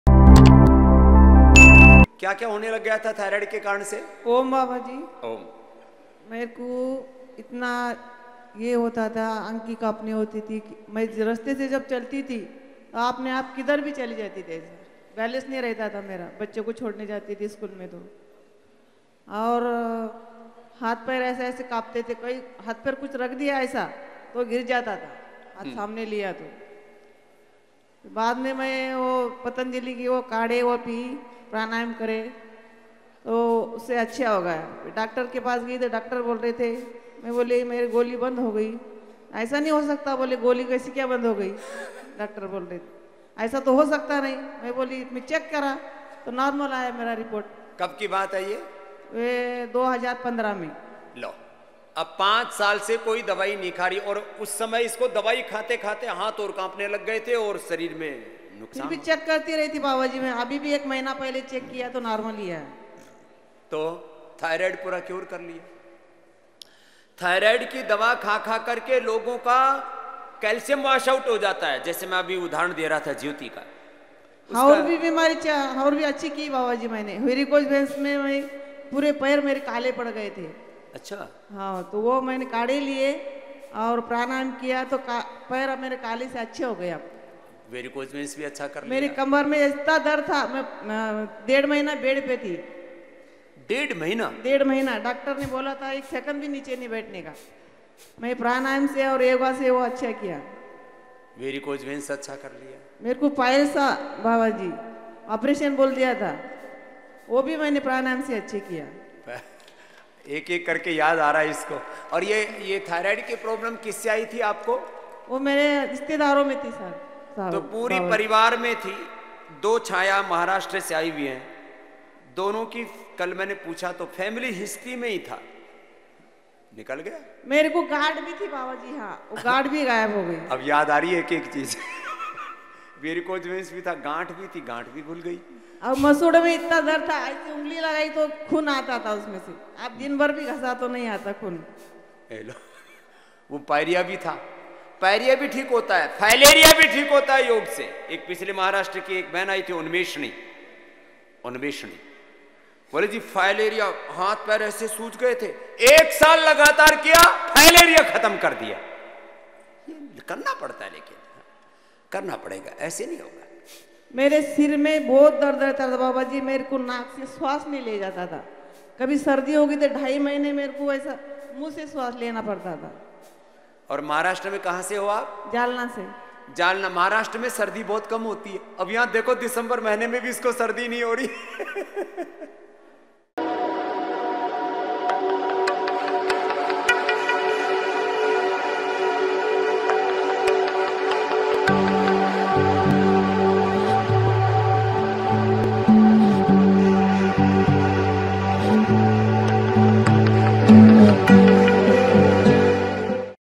थायराइड (Thyroid) के कम्प्लीकेशन को योग से किया ठीक || Swami Ramdev
Thyroid complications were cured by yoga  || Swami Ramdev

ओम बाबा जी।
ओम।  क्या-क्या होने लग गया था थायराइड के कारण से  ?
मुझे थायराइड की वजह से हाथ-पैरो में कंम्पन होने लगी थी। बैलेंस बिगड़ने लगा था। ठीक से चल नहीं पाती थी। हाथो में कुछ पकड़ नहीं पाती थी। उसके बाद मैंने प्राणायाम किया और पतंजलि के काढ़े पिये तो अब मैं काफी ठीक हूँ। मैंने डॉ को बताया की मेरी सब दवाइयां बंद हो गयी हैं, तो उन्होंने कहा की ऐसे कैसे हो गया। मैंने चेक करवाया तो सब नार्मल आया।
कब की बात है ये ?
जी 2015 में हुआ था।
अब देखो पांच साल से कोई दवाई नहीं खा रही है और उस समय दवाई खाते-खाते इसके हाथ पैर कांपने लगे थे।
बाबा जी मैं चेक करती रहती हूँ अभी भी करवाया है बिलकुल नार्मल आया है।
थायराइड की दवा खा-खा कर लोगों का कैल्शियम कम हो जाता है।
स्वामी जी मैंने और भी बीमारियां ठीक की हैं। वेरीकोस से मेरे पूरे पैर काले हो गए थे मैंने काढ़े पिए और प्राणायाम किया,अब मेरे पैर ठीक हो गए हैं। मेरे कमर में बहुत दर्द था मैं डेढ़ महीना बिस्तर पर थी। डॉ ने मुझे नीचे बैठने को मना किया था। मैंने प्राणायाम और योग से कमर दर्द भी ठीक किया। मुझे पाईल्स की समस्या थी उसे भी मैंने प्राणायाम और योग से ठीक किया। मेरा सर दर्द भी प्राणायाम से ठीक हुआ।

OM, baba JI
Om. What started happening due to thyroid?
I started having tremors in my hands and feet due to my thyroid. The balance was starting to deteriorate. Couldn't walk properly. Couldn't hold anything in my hands. After that I did Pranayama and drank Patanjali decoction, so now I am quite fine. I told the doctor that all my medicines have stopped, then he said how did this happen. When I got checked, everything came normal.
When did this happen?
It happened in 2015.
Now look, no one is taking medicine for five years and at that time his hands and feet started trembling while eating medicine.
Babaji, I keep on checking, even now I have got it done, it is completely normal.
People's calcium gets reduced by taking thyroid medicine.
Swamiji, I have cured other diseases. My whole legs had become black due to varicose, I drank decoction and did pranayama, now my feet are fine. There was a lot of pain in my back, I was in bed for one and a half months, the doctor forbade me to sit down. I also cured back pain with pranayama and yoga. I had the problem with Piles, I did that too with Pranayama and Yoga. My headache also got cured with Pranayama.